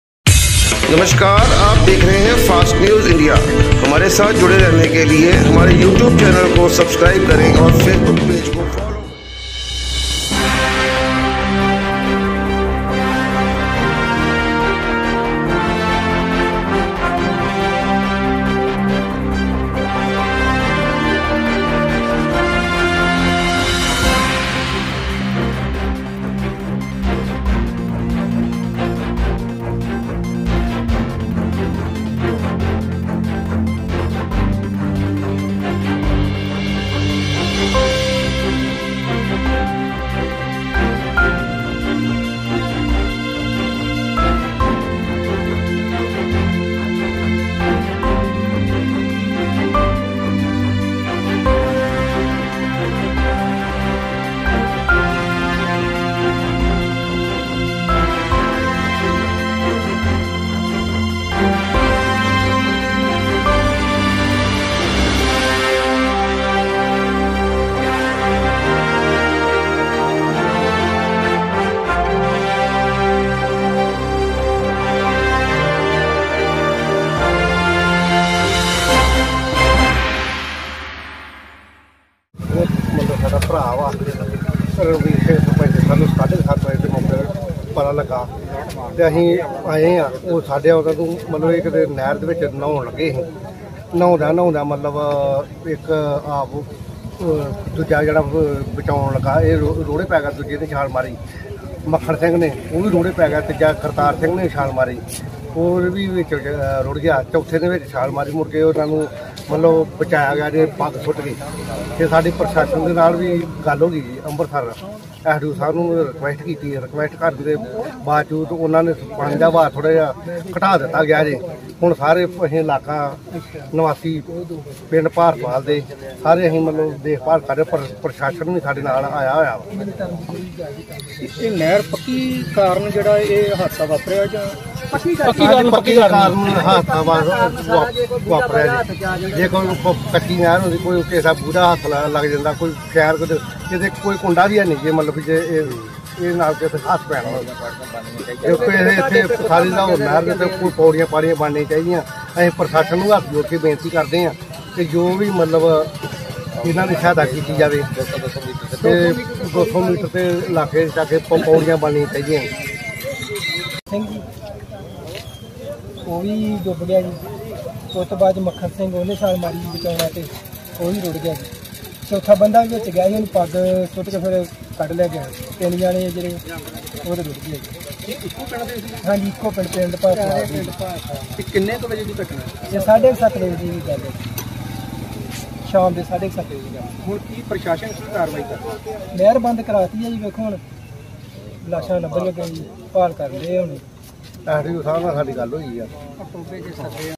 अपरबारी दोआब नहर में नहा रहे सरपंच समेत तीन डूब गये@FASTNEWSINDIA1965
अपरबारी दोआब नहर में नहा रहे सरपंच समेत तीन डूब गये